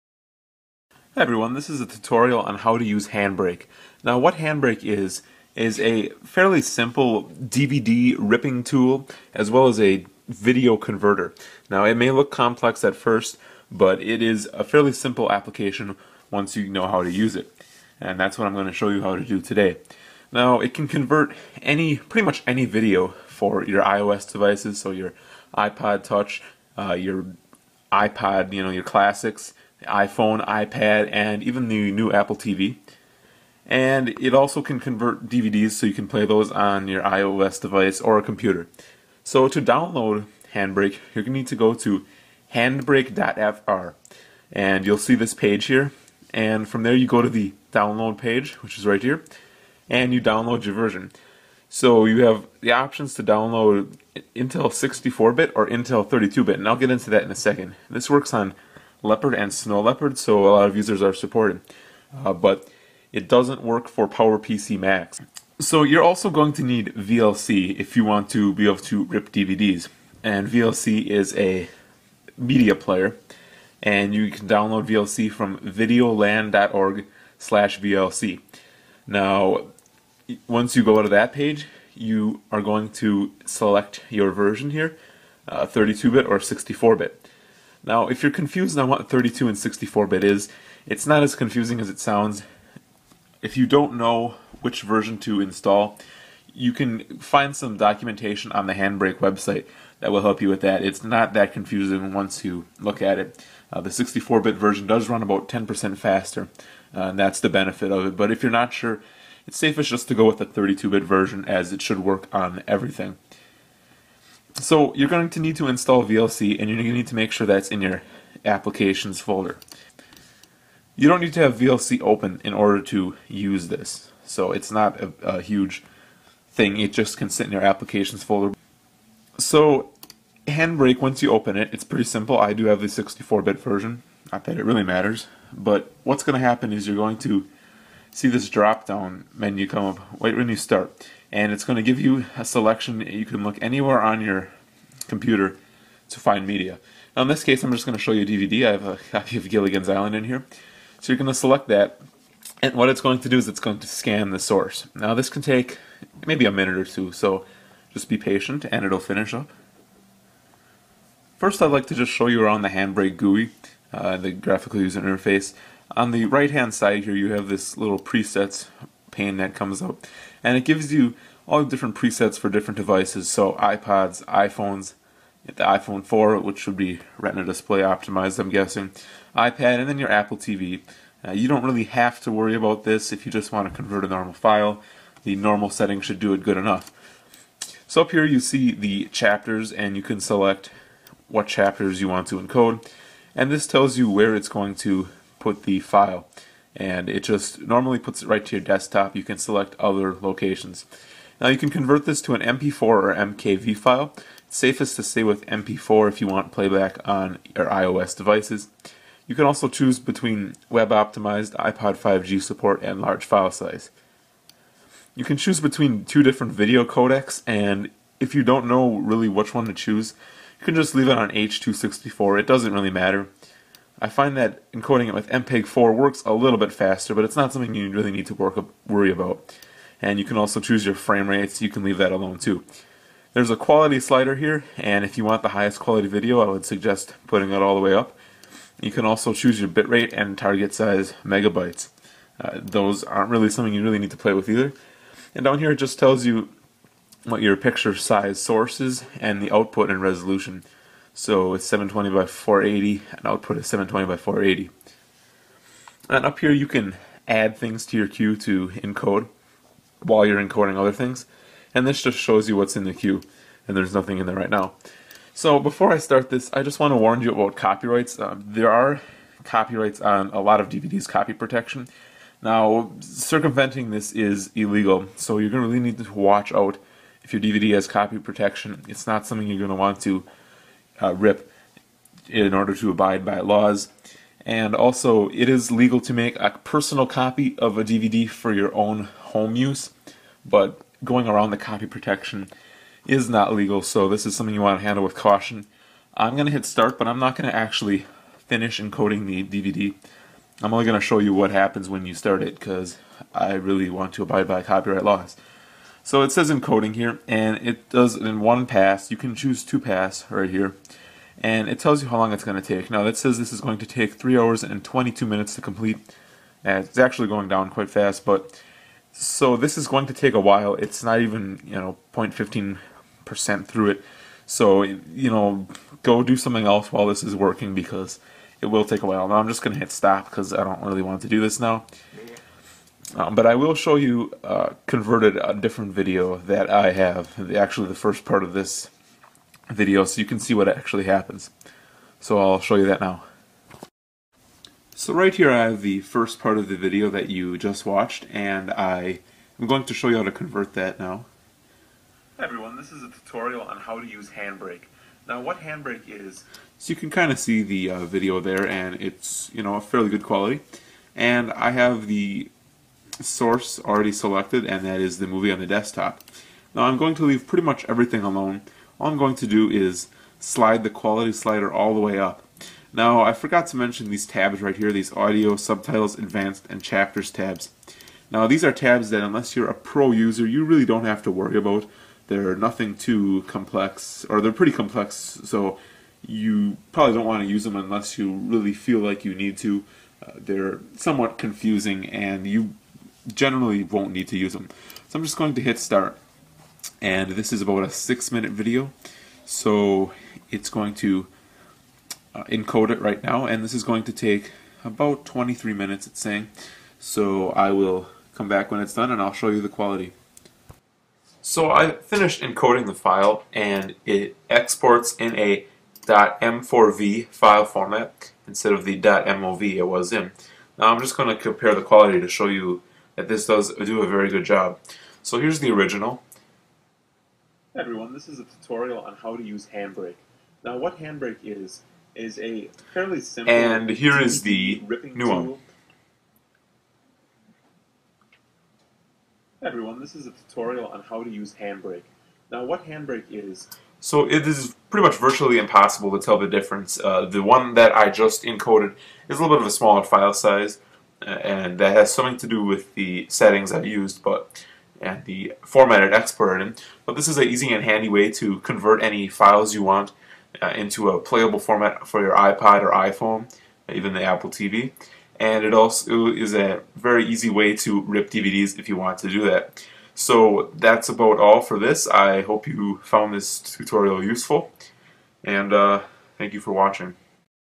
How to convert DVD's with Handbrake
A tutorial on how to convert any DVD or video file into a playable format for iPhones, iPods, iPads, and Macs.